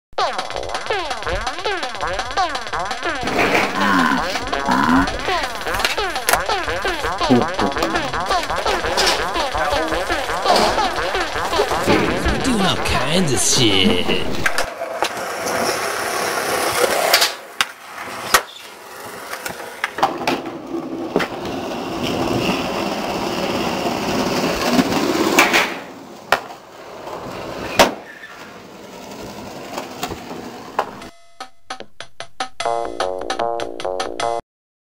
NOLLIE CAB off bank- clip of the day...free park montage coming soon
MONTAGE COMING SOON
NOLLIE CAB OFF BANK
feautureing phat pile team rider X-MAN  xavier dolida